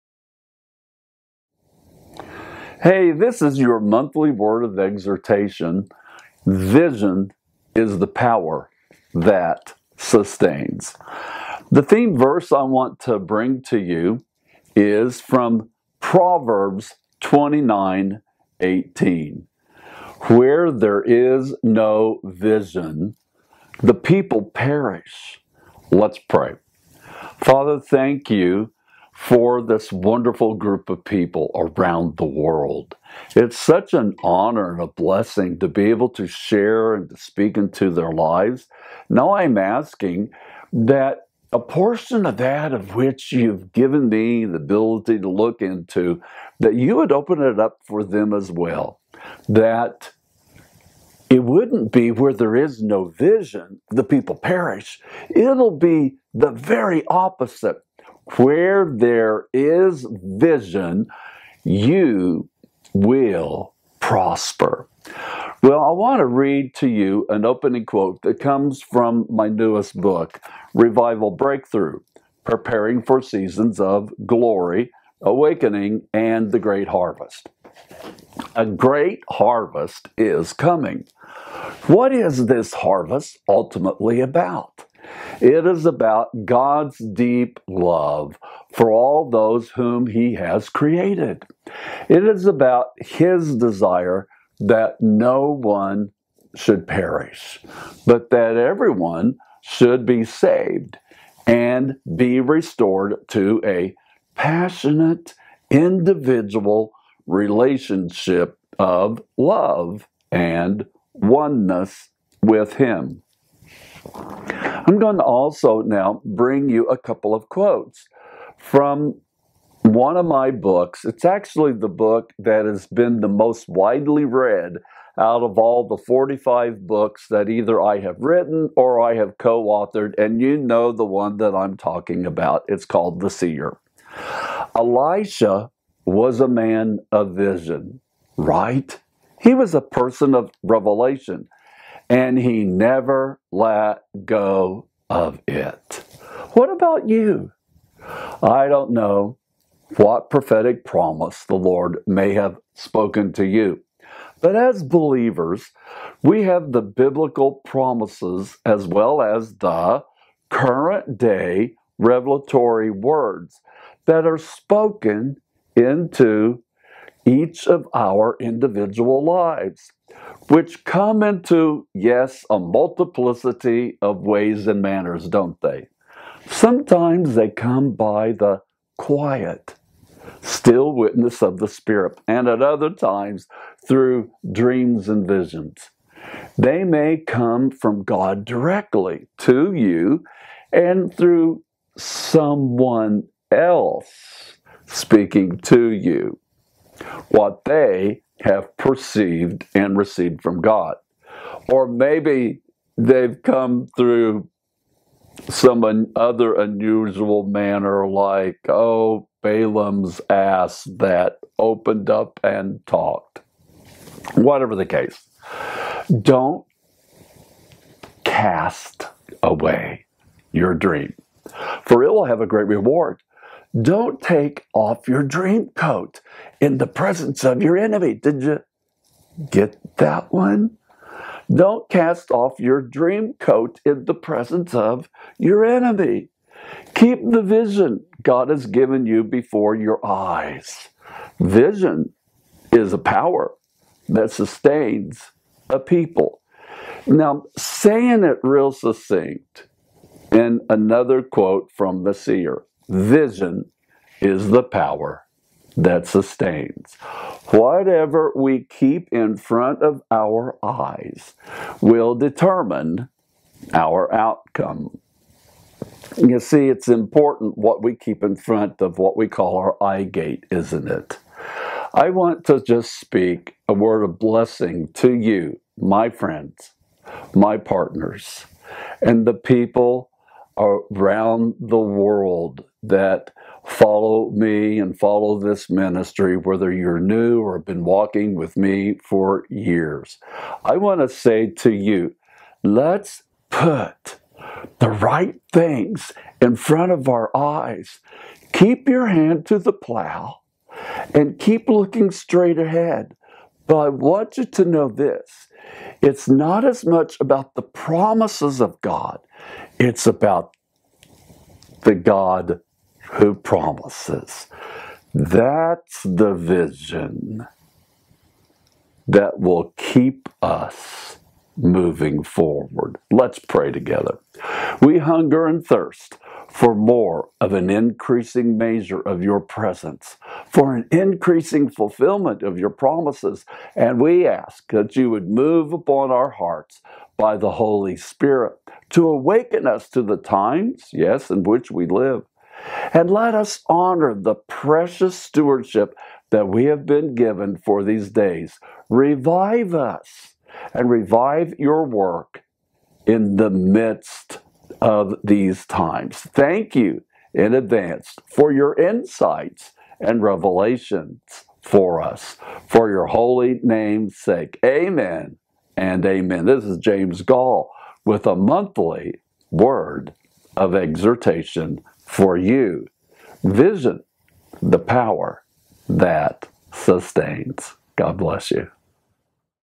A Great Harvest Is Coming! | James Goll
A great harvest is coming! What is this harvest ultimately about? It is about God's deep love for all those whom He has created. It is about His desire that no one should perish but that everyone should be saved and be restored to a passionate relationship of love and oneness with Him.

If this video has blessed you and you'd like to partner with us financially, you can donate